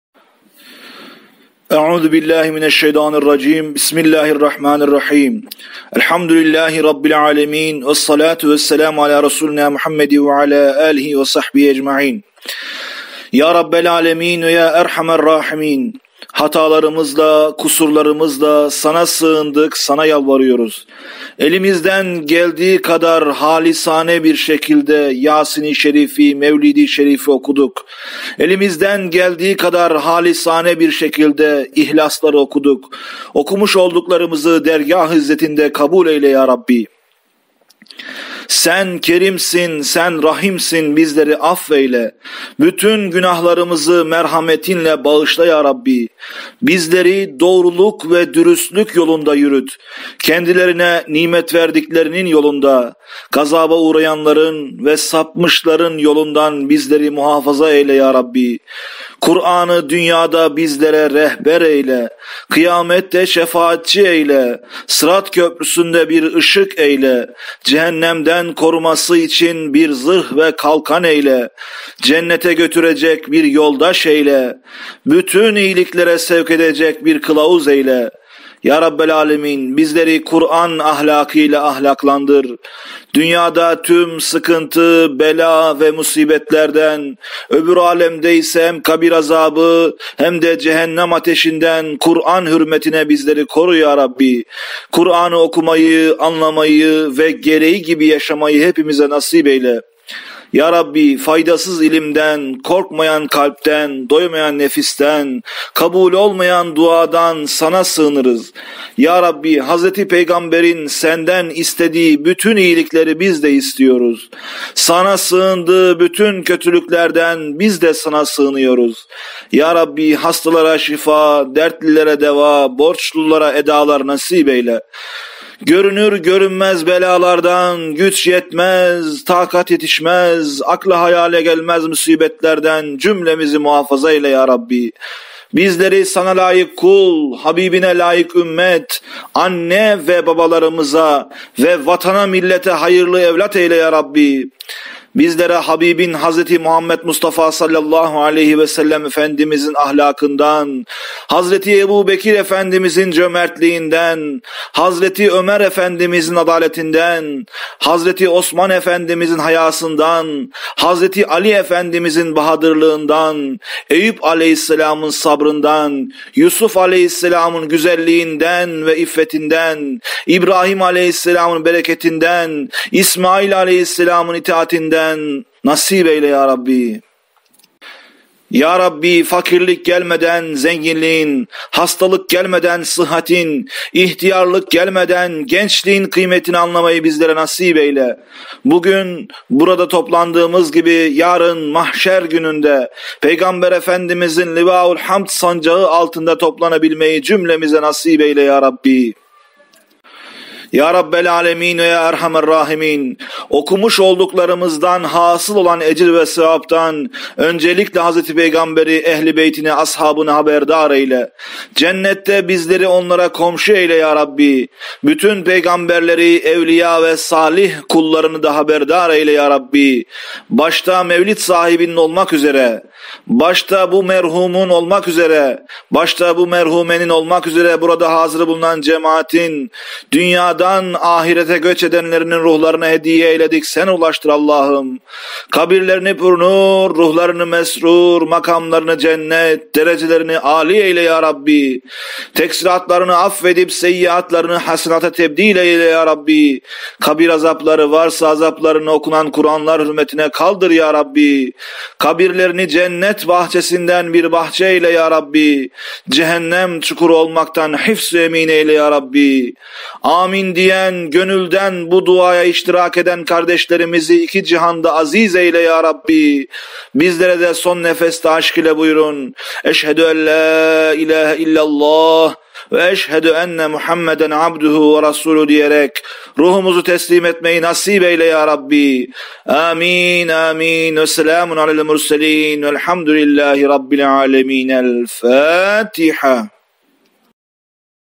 Mevlid Hatim Cenaze Duası (metinli)
Mevlid Hatim Cenaze Duası
Ali Rıza Özpınar

Ya rabbel alemin ve ya erhamerrahimin!

       Hatalarımızla kusurlarımızla sana sığındık sana yalvarıyoruz. Elimizden geldiği kadar halisane bir şekilde mevlidi/yasini/ihlasi şerifi okuduk. okumuş olduklarımızı yüce katında kabul eyle.

       Sen Kerimsin sen Rahimsin bizleri affeyle; bütün günahlarımızı merhametinle bağışla yarabbi.
        Bizleri doğruluk ve dürüstlük yolunda yürüt, kendilerine nimet verdiklerinin yolunda gazaba uğrayanların ve sapmışların yolundan bizleri muhafaza eyle yarabbi.

         Ey rabbimiz Kuranı, dünyada bizlere rehber eyle. Kıyamette şefaatçi eyle. Sırat köprüsünde bir ışık eyle. Cehennemden koruması için bir zırh ve kalkan eyle. Cennete götürecek bir yoldaş eyle. Bütün iyiliklere sevk edecek bir kılavuz eyle.

        Ya rabbi Bizleri kuran ahlakıyla ahlaklandır. Dünyada tüm sıkıntı bela ve musibetlerden öbür alemde ise hem kabir azabı hem de cehennem ateşinden kuran hürmetine bizleri koru yarabbi.
       Ya rabbi Kuranı okumayı, anlamayı ve gereği gibi yaşamayı hepimize nasip eyle.

       Allahım Faydasız ilimden, korkmayan kalpten, doymayan nefisten ve kabul olmayan duadan sana sığınırız.
      Allahım Hz. Muhammed’in senden istediği bütün iyilikleri biz de istiyoruz, sana sığındığı bütün kötülüklerden bizde sana sığınıyoruz.

       Ya rabbi Hastalara şifa, dertlilere deva, borçlulara edalar nasip eyle. Görünür görünmez belalardan, güç yetmez, takat yetişmez akla hayale gelmez musibetlerden cümlemizi muhafaza eyle.
      Ya Rabbi! Bizleri sana layık kul, habibine layık ümmet, anne ve babalarımıza hayırlı evlat eyle!
 Ya Rabbi! Bizlere habibin Muhammed Mustafa (sav)in ahlakından,
Hz. Ebu Bekir efendimizin cömertliğinden,
Hz. Ömer efendimizin adaletinden,
Hz. Osman efendimizin hayâsından,
Hz. Ali efendimizin bahadırlığından,

      Eyüp as’ın sabrından,
     Yusuf as’ın güzelliğinden ve iffetinden,
     İbrahim as’ın bereketinden,
     İsmail as’ın itaatinden ihsan eyle!

    Ya Rabbi Fakirlik gelmeden zenginliğin, hastalık gelmeden sıhhatin ve ihtiyarlık gelmeden gençliğin kıymetini anlamayı nasip eyle!
      Bu gün buraya toplandığımız gibi yarın mahşer gününde peygamber efendimizin ‘liva-ül hamd’ sancağı altında toplanabilmeyi cümlemize nasip eyle!

          Okumuş olduklarımızdan hasıl olan ecir ve sevaptan öncelikle hz. Peygamberi ehli beytini ashabını haberdar eyle cennette bizleri onlara komşu eyle. Bütün peygamberleri evliya ve salih kullarını da haberdar eyle ya rabbi.

          Başta mevlit sahiplerinin/ bu merhumun olmak üzere burada hazır bulunan cemaatin dünyadan ahirete göç edenlerinin ruhlarına hediye eyledik sen ulaştır Allahım. Kabirlerini pür nur, ruhlarını mesrur, makamlarını cennet, derecelerini ali eyle.
          Taksiratlarını af edip seyyiatlarını hasenata tebdil eyle.  kabir azapları varsa  azaplarını okunan kuran hürmetine kaldır ya Rabbi!. Kabirlerini cennet bahçesinden bir bahçe eyle cehennem çukuru olmaktan hıfzu emin ey ya rabbi.
           Âmin diyen, gönülden bu duaya iştirak eden kardeşlerimizi iki cihanda aziz eyle yarabbi.
          Bizlerede son nefesimizde: 'eşhedü ella ila heillallah ve eşhedü enne muhammeden abduhu ve rasuluhu' diyerek ruhumuzu teslim etmeyi nesip eyle yarabbi amin amin.